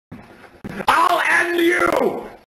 I'll end you!! (READ DESCRIPTION)
I made this way back in November of 2020, but was never released publicly. If I can recall we were filming episode 2 and the scene about to be filmed was were James tries to apologize to Thomas in which he refuses to accept the apology. I made this because of how Thomas was so pissed off it seemed to me he would say I'll end you!, hence the name of the video of the screenshot I took when filming. I also found more behind the scenes photos of Episode 2's filming, if I can remember I wanted to make a BTS video of it but it never worked out. But enjoy this lol.